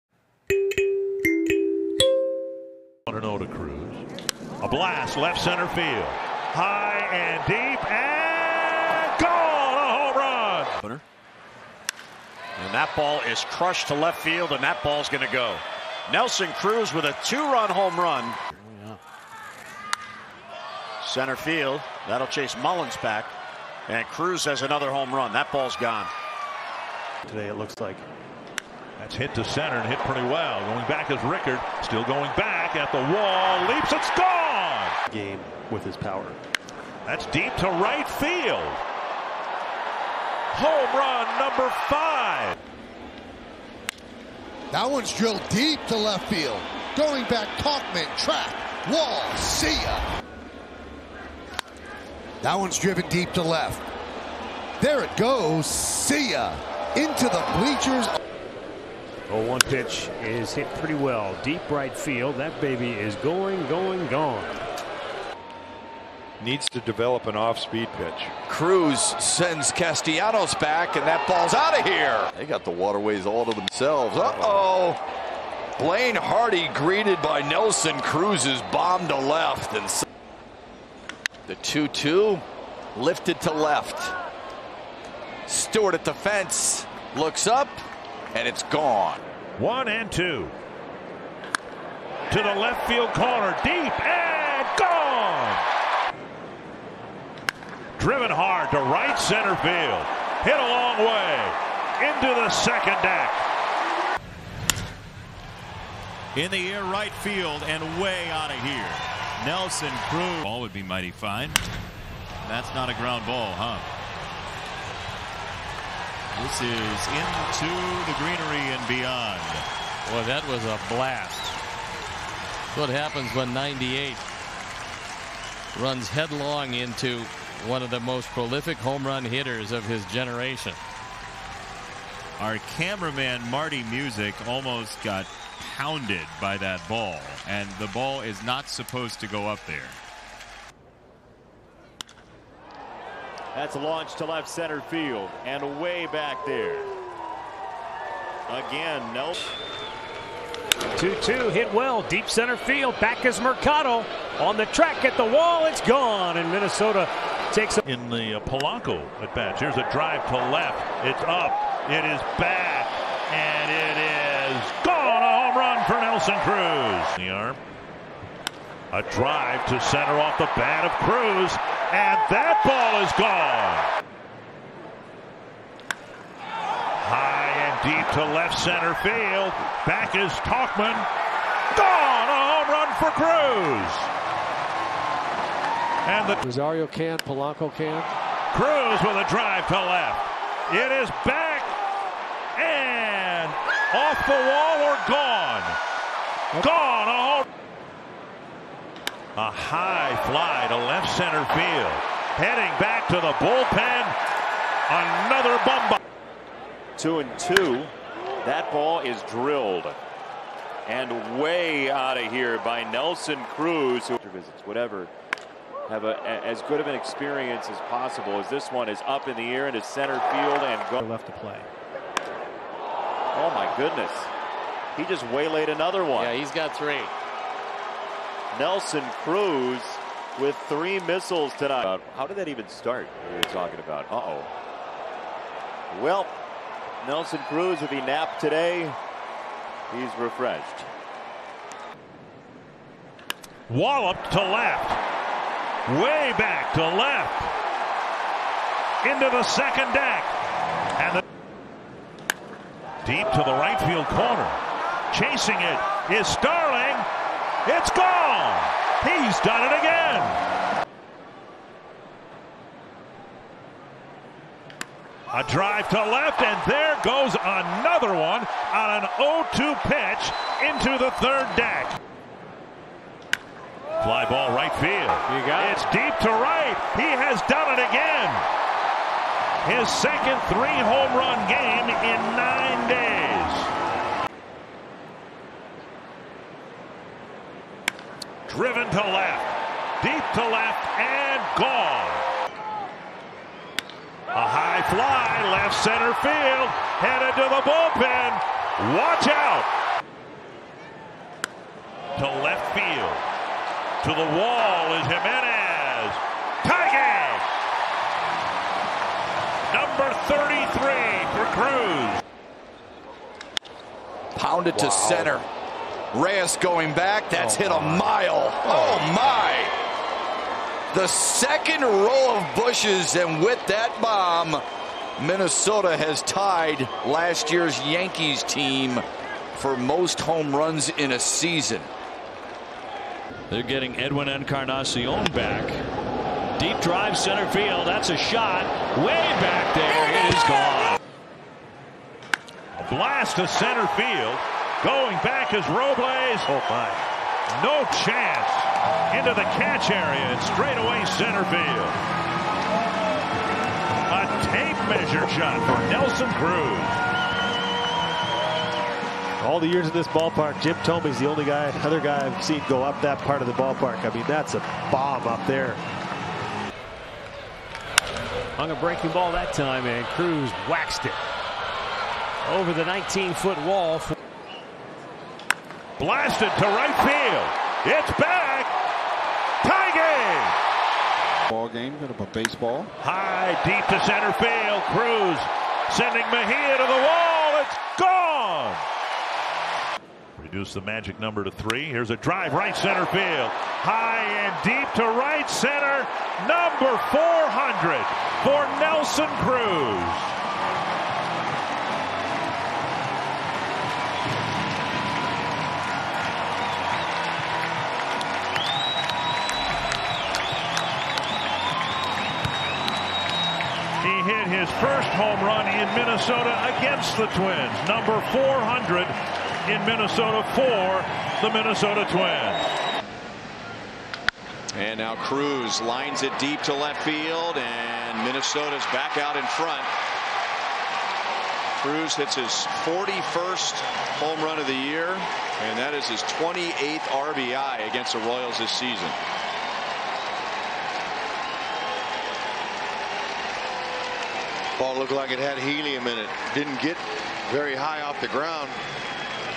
Twins Highlights: Nelson Cruz's 2019 Home Runs
The Minnesota Twins established a new home run record by blasting 307 bombas in 2019. Here are all 41 of Nelson Cruz's home runs on the season.